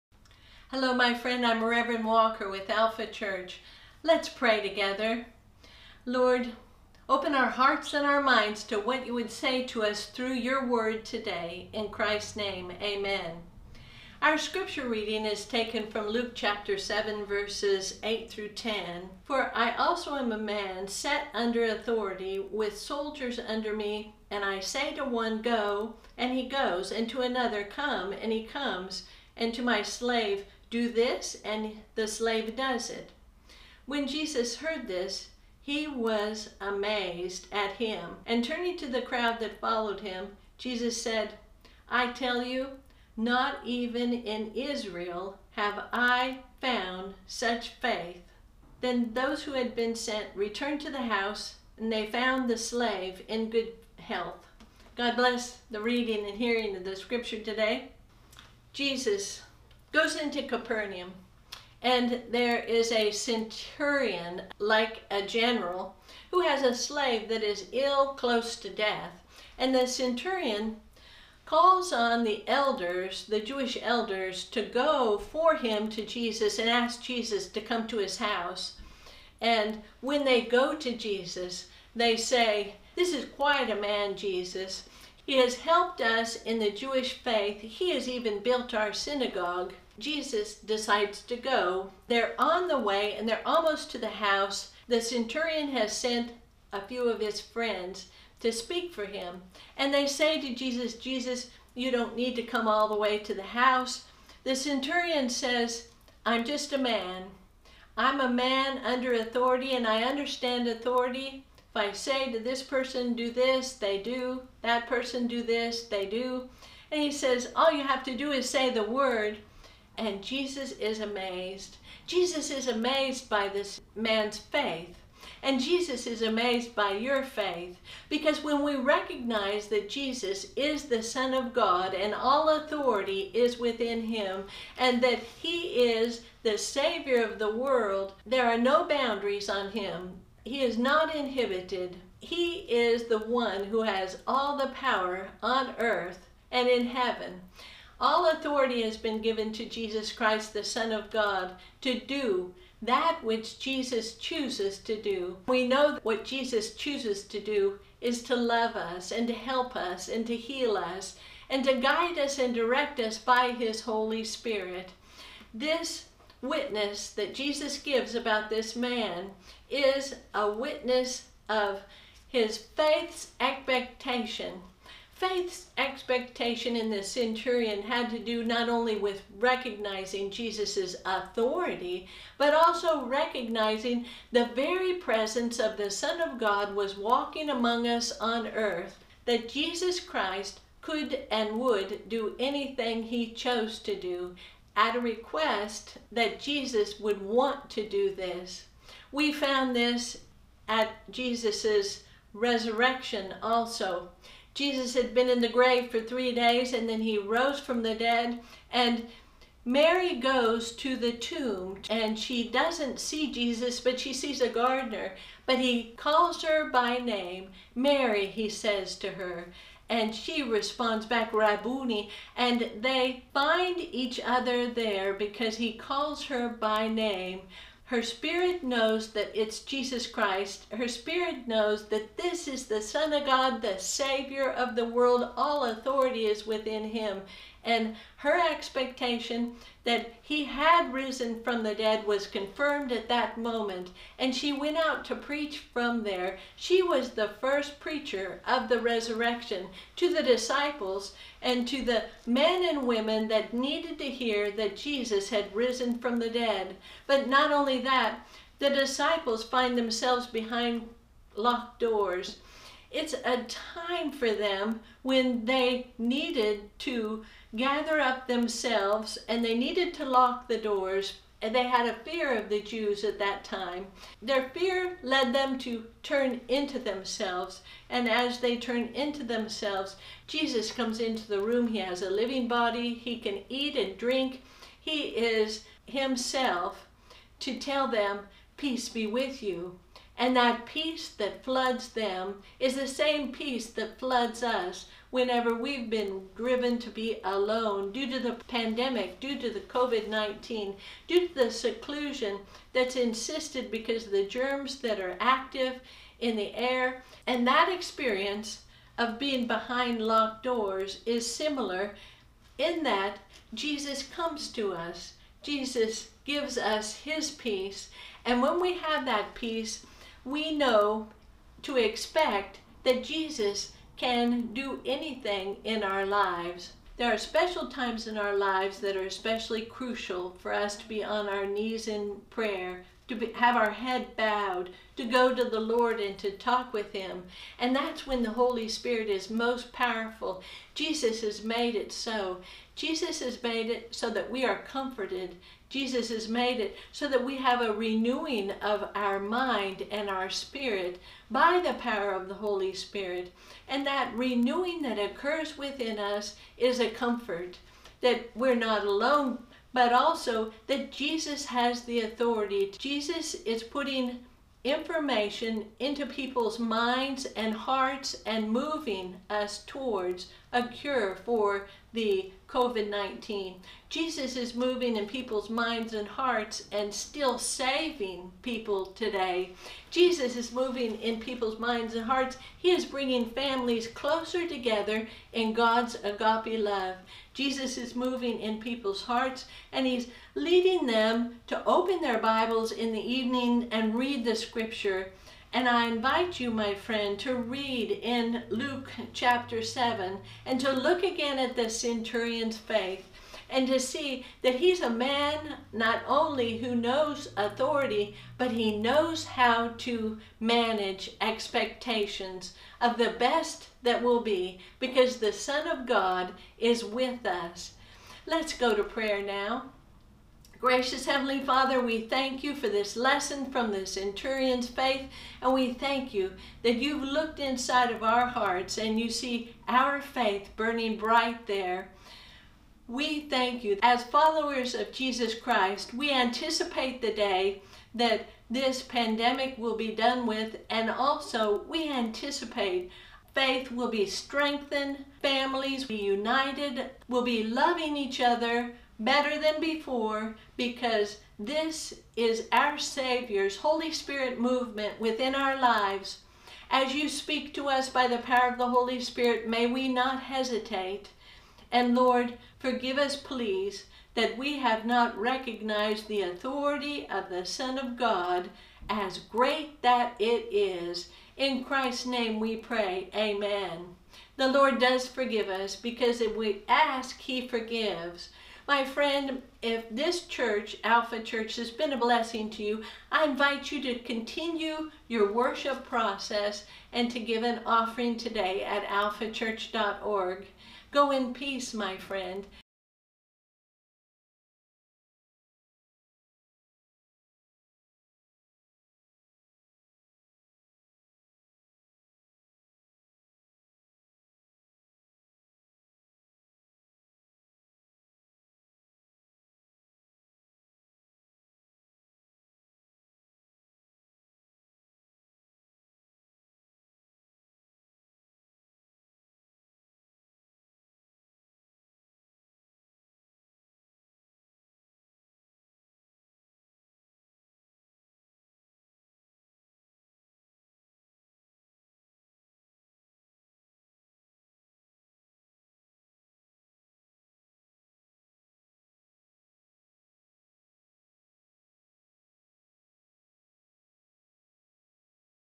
How To Manage Expectations During COVID-19 Pandemic
A Centurion sends a delegation to Jesus requesting Him to come and heal his ill near death servant.  Yet on the way to him something remarkable happens and Jesus responds by saying,  “I tell you, not even in Israel have I found such faith.”  (Luke 7:9)  Your faith during COVID-19 is shown in how you manage expectations, like the Centurion shows us and is shown others in New Testament Scripture.
A "How=To' message, faith in Christ Jesus, Luke 7